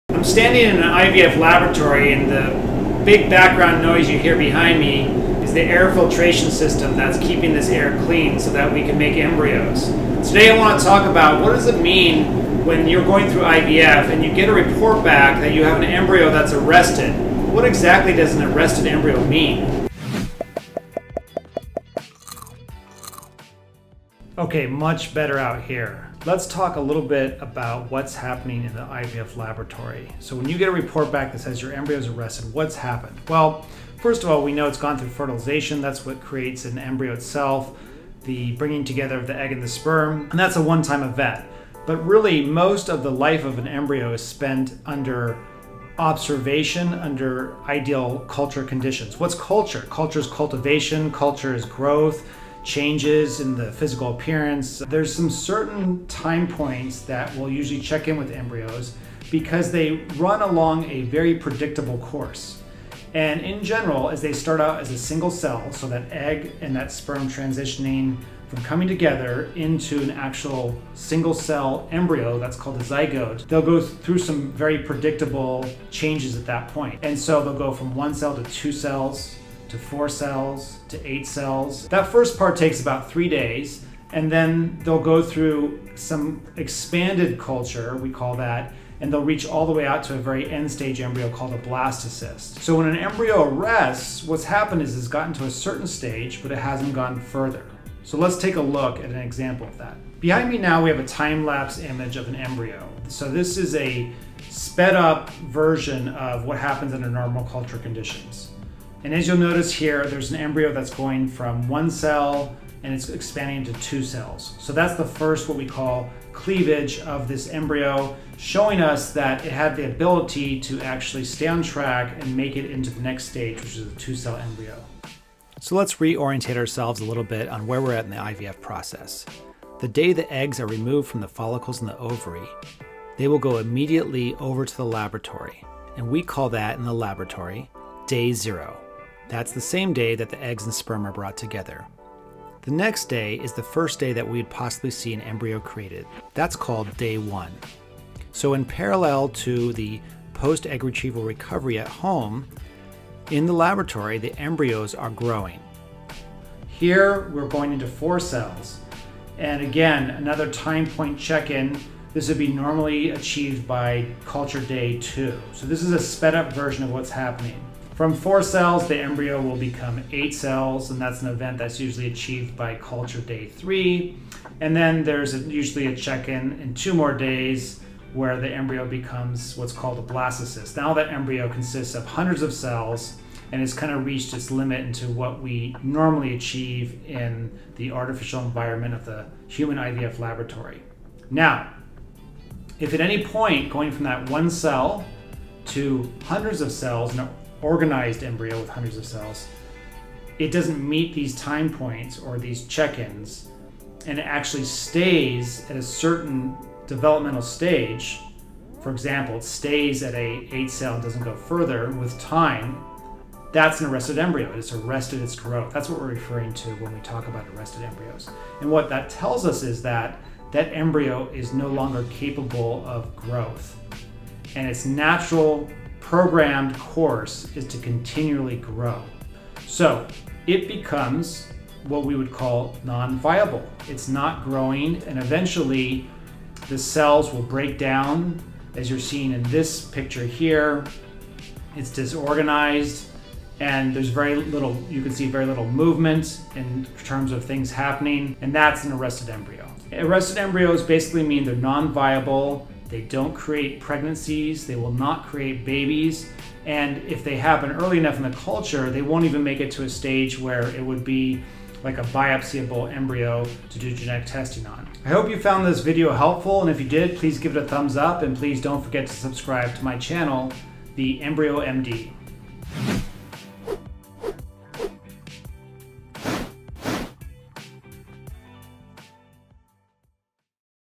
What is an arrested embryo?
The embryo md explains what it means when a human IVF laboratory reports to a patient that an embryo has "arrested" during culture.